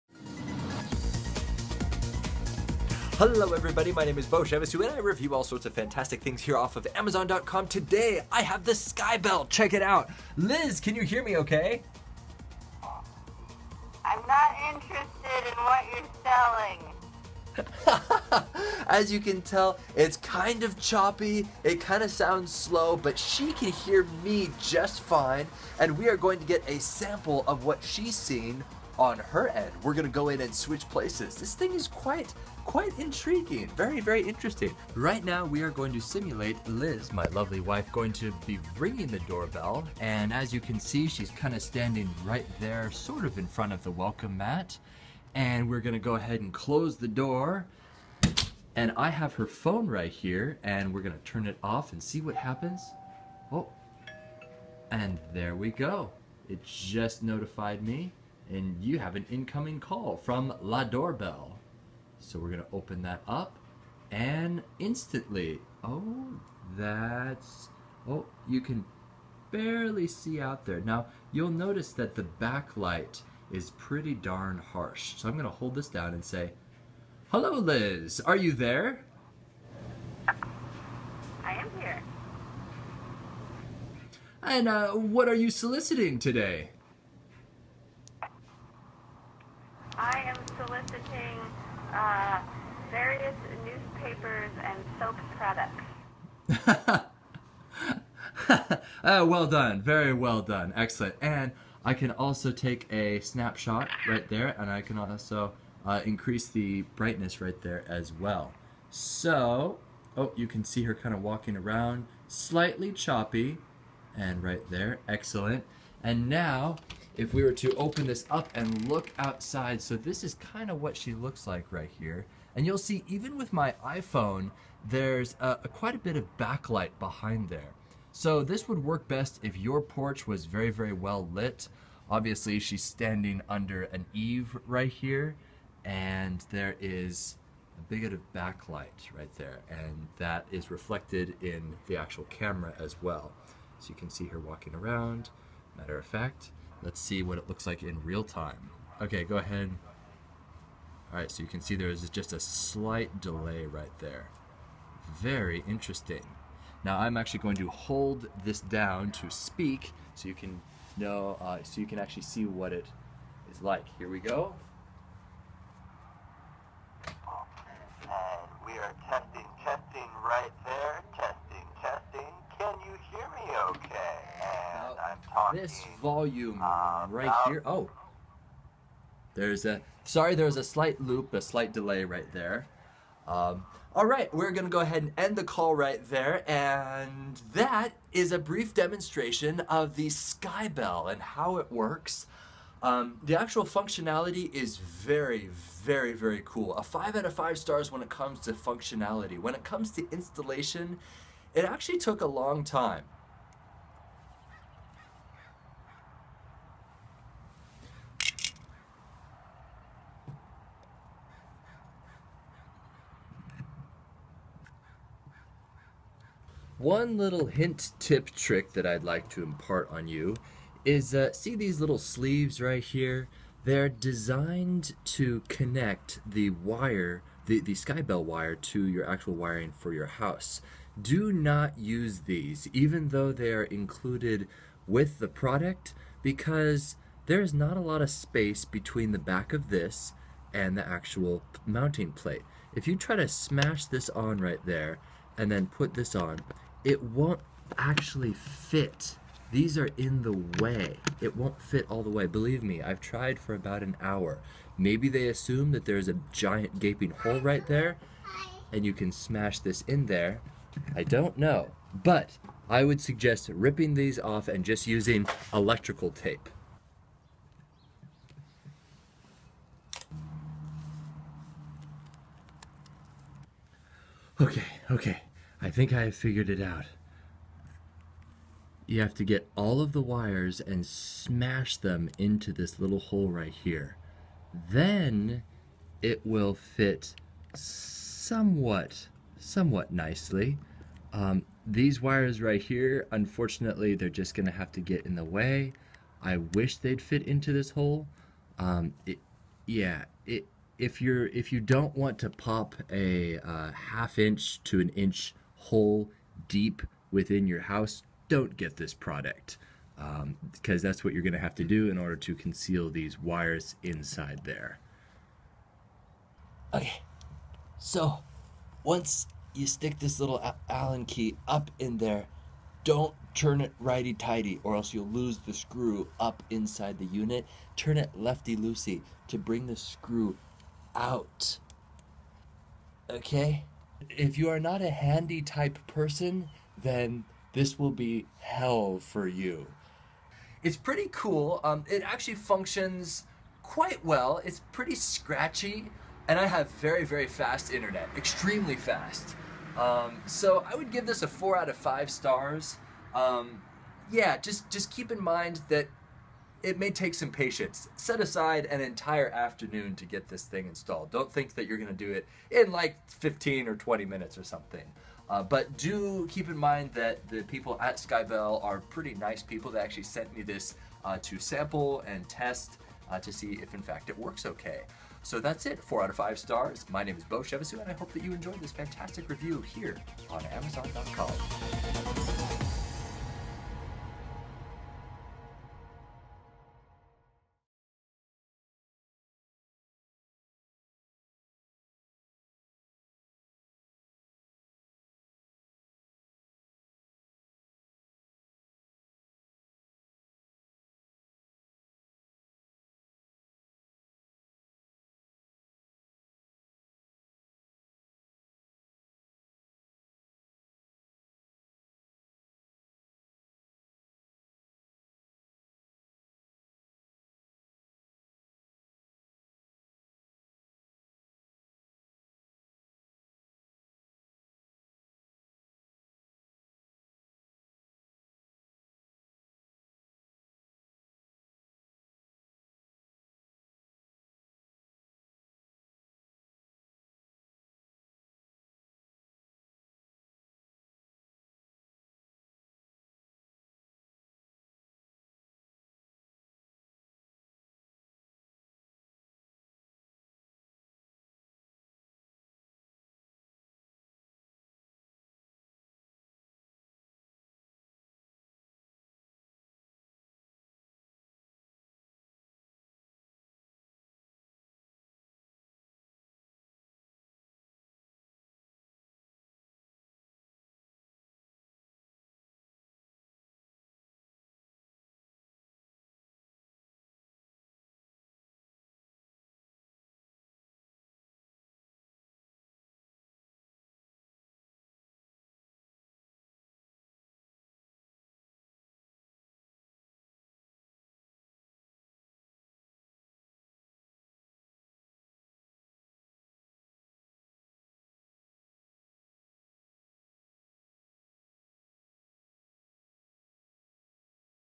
★★★★☆ SkyBell HD Installation & Review - Wi-Fi Video Doorbell Version 2.0 - Amazon
🎥 Video review. 🎥
I use this on my covered porch here in the Pacific Northwest.  This is an excellent gadget for any family to have, but do note it works best if you have a smart phone, are technically advanced for doing advanced configuration on a Wifi router, are handy with tools, know electrician jargon, very patient, and can afford the price.

INSTALLATION:  While it seems cut-and-dry, this is NOT an easy 15 minute project... I would recommend setting aside an entire afternoon.

Pros:
✅ Awesome idea for a doorbell: I've told my wife before, "If I'm not home, please don't answer the door!"  (there are creepy people out there)
✅ Looks nice on my house.
✅ Nice folks at SkyBell. (they actually sent me a sample to review and test)
✅ Fairly quick bootup of the app when someone rings the doorbell. (tested using iPhone 5 and iPhone 5s)
✅ Our original door bell chime still works; that is my phone "rings" when someone pushes the button AND our normal bell chimes. (the chime now sounds a little funky... it sort of pauses a really long time between the "Ding" and the "dong.")
✅ Free app. This may seem like a silly "pro" but yes, there are companies out there who make you pay $10 for their app after you've purchased their product.

Cons:
🍅 Poor design with included wire "insulators."  The insulators are so thick, it prevents the SkyBell from mounting against the plate... even when the insulators are smashed down with a pair of pliers with all my might.
👉 NOTE: If you are willing to drill a 1" inch hole in your house to stuff all the wiring in, then the SkyBell will mount.  If you aren't willing to do this to your house, then my suggestion would be NOT to purchase it.
🍅 Scratchy audio and choppy video (not something you'd expect from a +$150 device). My internet is blazingly fast and the Wifi router is actually just 10 feet away from the SkyBell.  Still, quite choppy.  See video for a demo of the footage.
🍅 If you have intense backlight (as I'm sure most porches would have), it makes the subject very hard to see.  (see video)  I've tested some cheap $70 dash cams with better resolution, color correction, and low-light abilities.
🍅 Confusing instructions: If you're a contractor, electrician, or have done remodeling, you'll probably be okay. But I'd wager the normal person (such as myself) doesn't really know what this means: "be sure you have a power supply with a low-voltage transformer with power from 10 to 36 VAC 10 VA or 12 VDC with an amp rate of 0.5 to 1.0." (❔❔❔❔)
🍅 If you can afford the price tag, more power to you.  In my humble opinion, I would not spend this much for what is provided. 💸
🍅 Setting up your Wifi router requires advanced technical know-how.  I personally was comfortable with setting it up, but I imagine 8/10 people buying this product would be very puzzled with setting up their wifi so it's "2.4GHz band."  Do you know what "b/g mode" is on your wifi router?  Can you switch it to "20 MHz bandwidth?"  I suppose this is why SkyBell emphasizes their free 24/7 tech support, which I did not experience but they probably get A LOT of telephone calls.

While it seems like a lot of "cons," really the overall idea of the device is quite awesome. It works as advertised for the most part, just as long as you are 1. tech savvy, 2. have some electrician knowledge, 3. can afford the price tag, 4. don't mind choppy video and poor audio, and 5. are very patient.